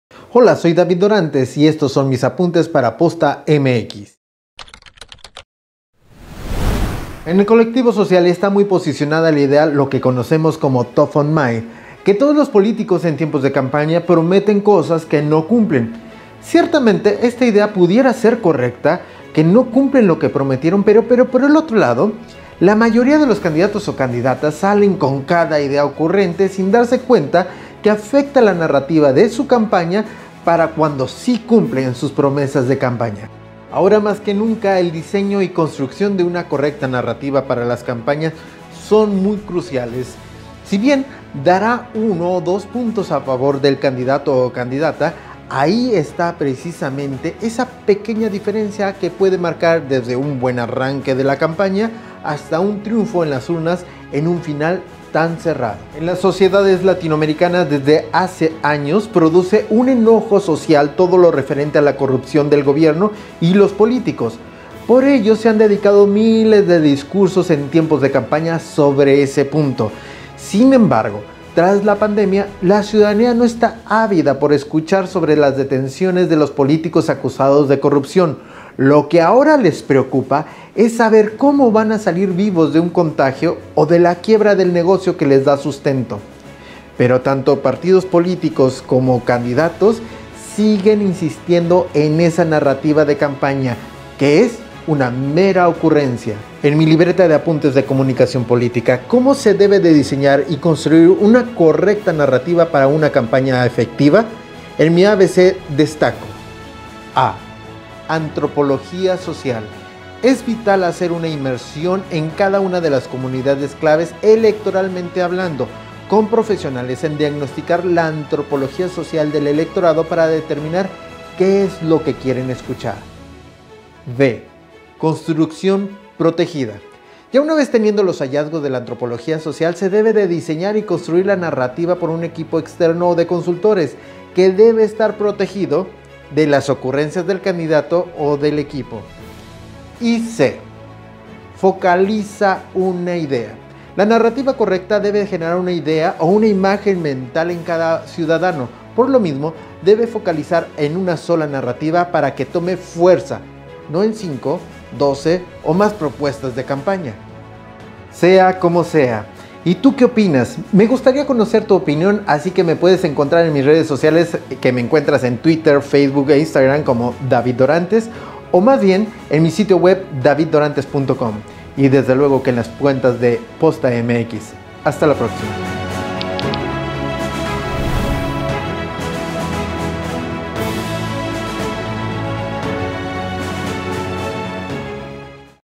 Afecta narrativa de campaña ocurrente
En el colectivo social está muy posicionada la idea –lo que conocemos como top of mind- que todos los políticos en tiempos de campaña prometen cosas que nunca cumplen, ciertamente es una idea correcta que no cumplen lo que prometieron, pero por otro lado la mayoría de los candidatos o candidatas salen con cada idea ocurrente sin darse cuenta que afecta la narrativa para su campaña aunque sí cumplan sus promesas.